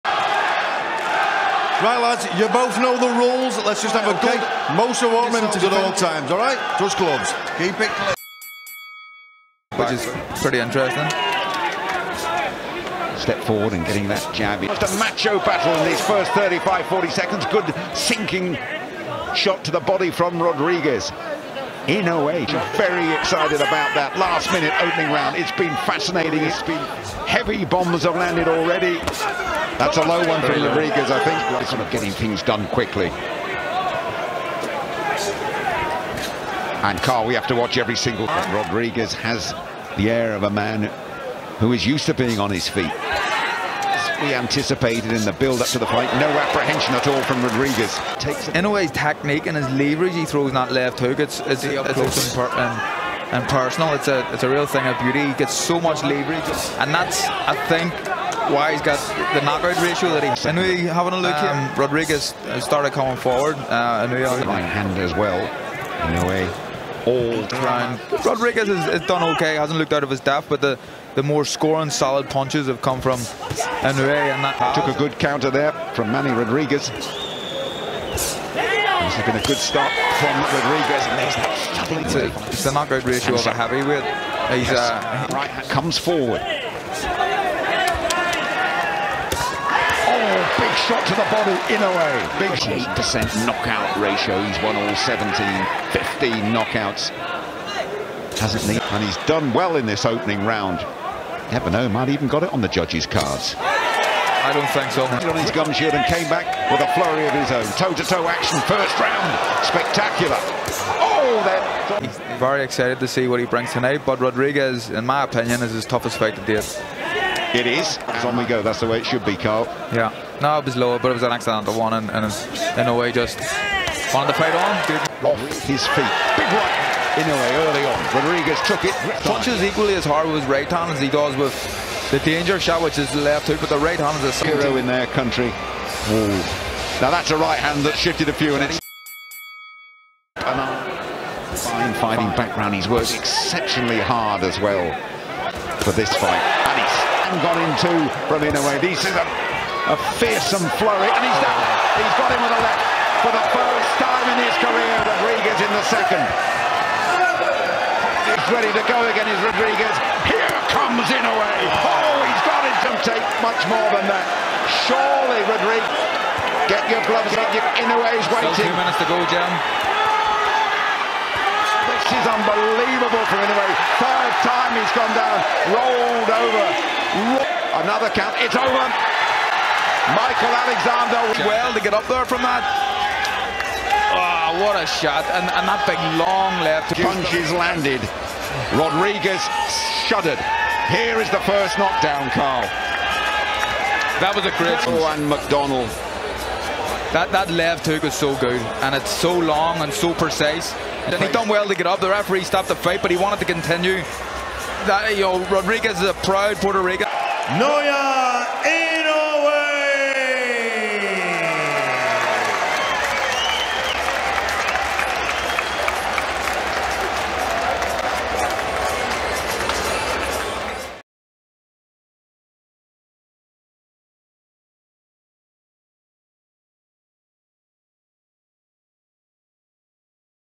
Naoya Inoue vs Emmanuel Rodriguez | Fight Highlights | BOXING FIGHT | HD
Watch the intense battle between Naoya "Monster" Inoue and Emmanuel Rodriguez in this highlight reel! Inoue, known for his explosive knockout power and lightning-fast hands, faces the undefeated Rodriguez for the IBF bantamweight title. From the start, the fight is a display of elite-level skill, with Rodriguez showing his resilience and sharp counters. However, Inoue's devastating body shots and precision striking soon overwhelm his opponent. The highlight video captures every critical moment of this thrilling bout, including Inoue's crushing knockdowns and relentless pressure. Witness how Inoue cements his status as one of the most feared punchers in the sport, delivering a knockout that leaves the crowd in awe.